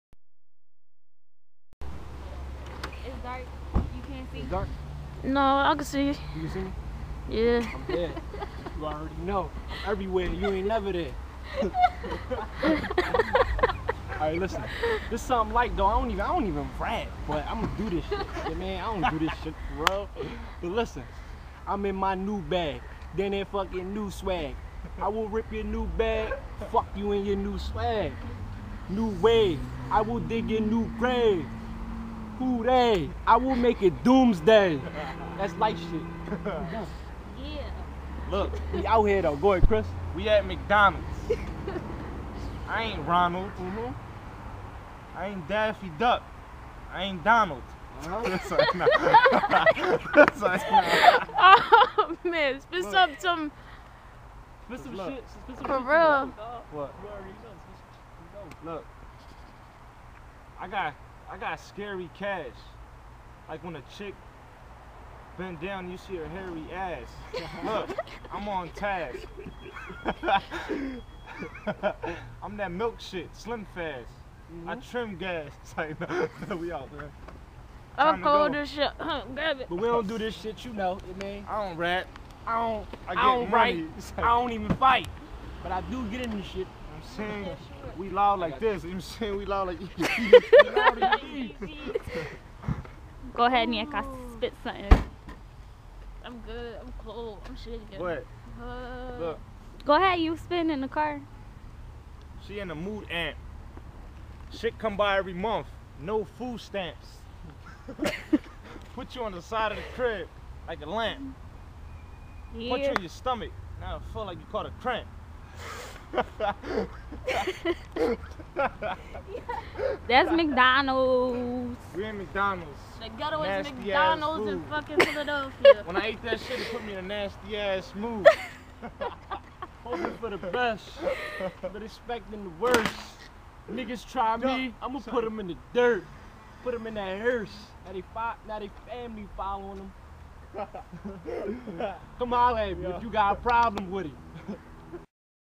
In Front of Mcdonalds
in front of mcdonalds buggin.. they rapping